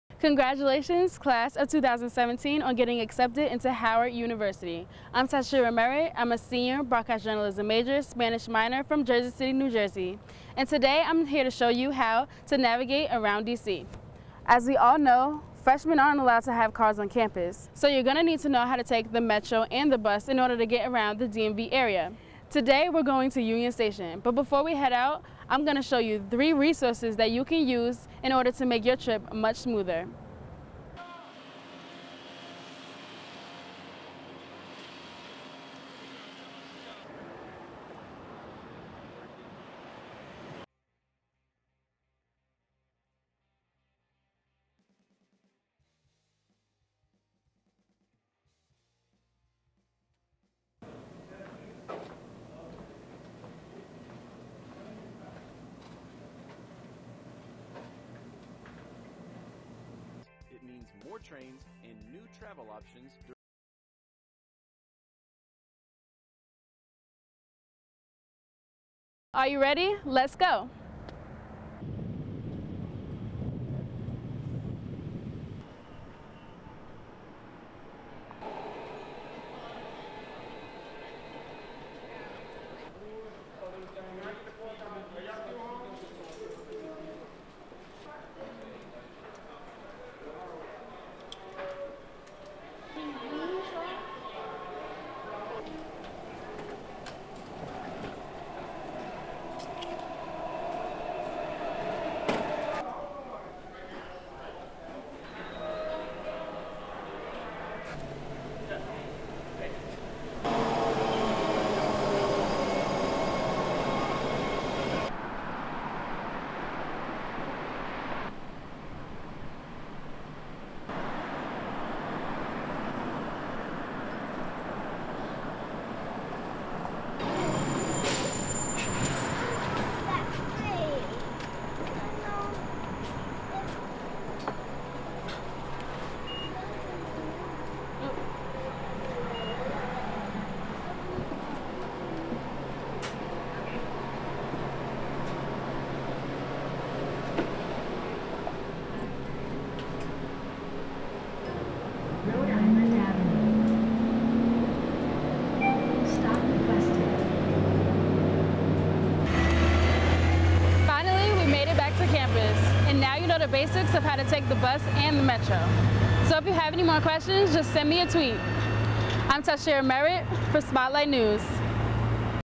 Metro
As the Class of 2017 gets acquainted with a new city, Spotlight Reporter Tashira Merritt shows us how to DC's public transportation! HU17 This is a must see!